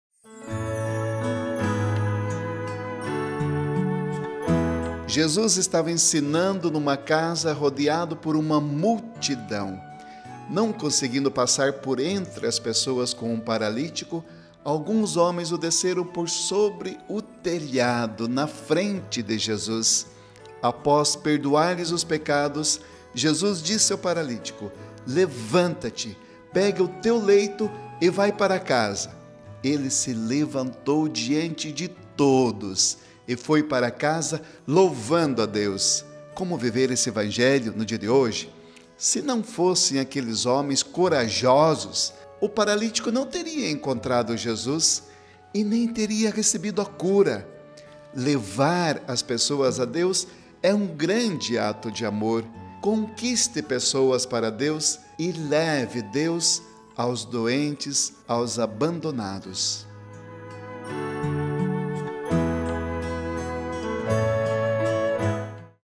Evangelho de 11 de dezembro
Jesus estava ensinando numa casa rodeado por uma multidão. Não conseguindo passar por entre as pessoas com um paralítico, alguns homens o desceram por sobre o telhado, na frente de Jesus. Após perdoar-lhes os pecados, Jesus disse ao paralítico: levanta-te, pega o teu leito e vai para casa. Ele se levantou diante de todos e foi para casa louvando a Deus.

Se não fossem aqueles homens corajosos, o paralítico não teria encontrado Jesus e nem teria recebido a cura. Levar as pessoas a Deus é um grande ato de amor. Conquiste pessoas para Deus e leve Deus aos doentes, aos abandonados.